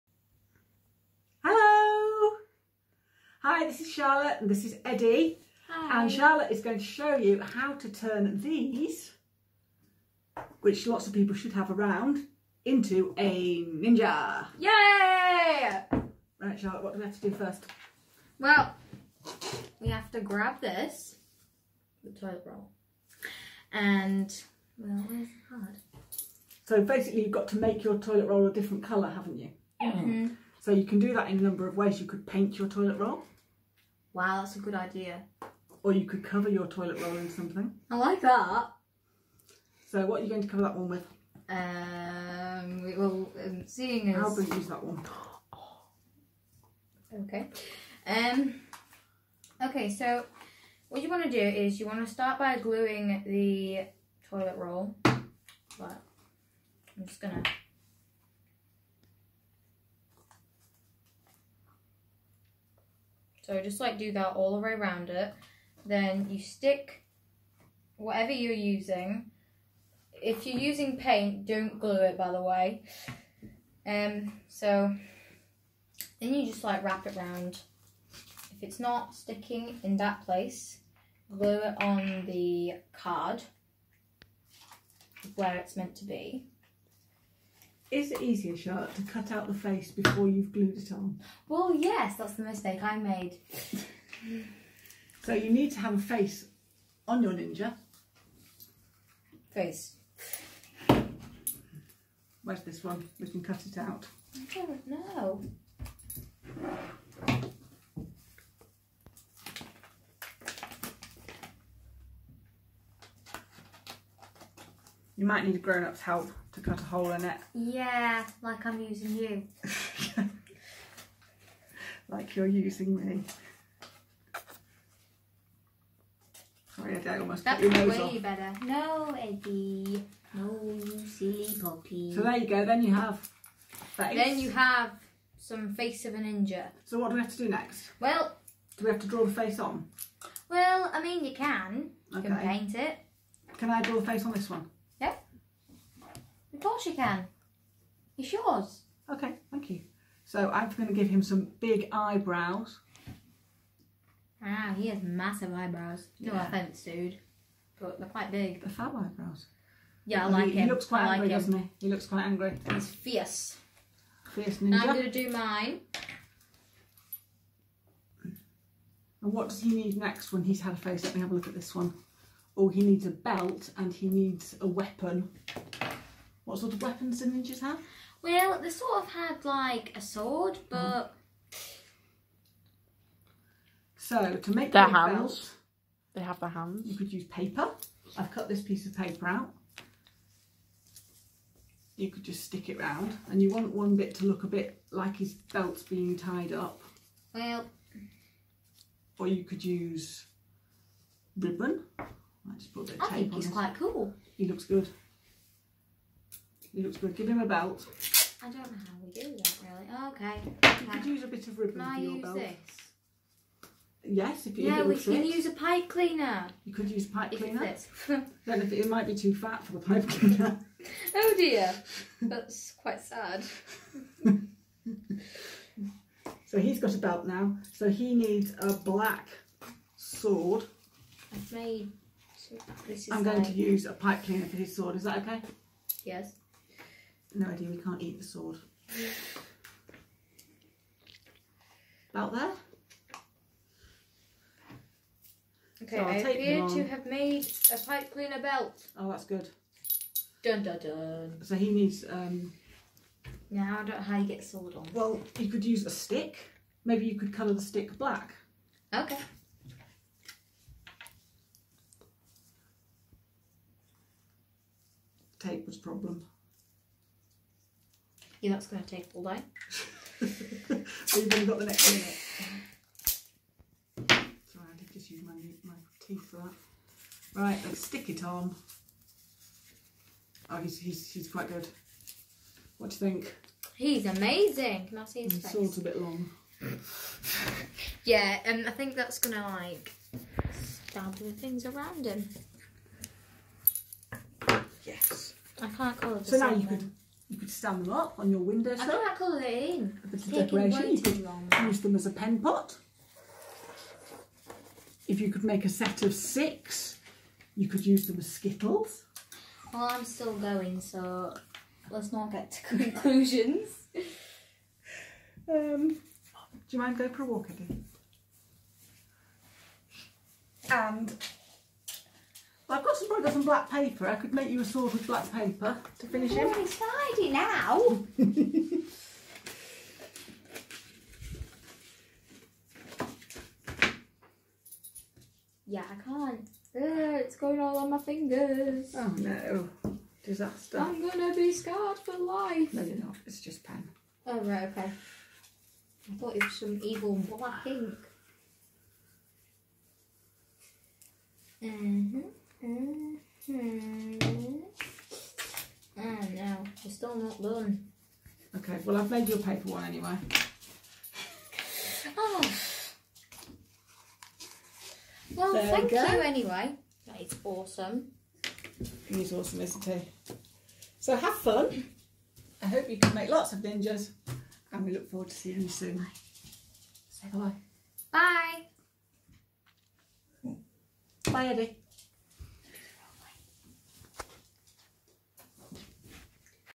Art- toilet roll ninjas!
Join Mes Lewis, Charlotte and Eddie the dog as they demonstrate how to make use of all those toilet rolls! Why not make a cool or fierce ninja?

What else could you make with a toilet tube?

Enjoy and keep smiling ☺️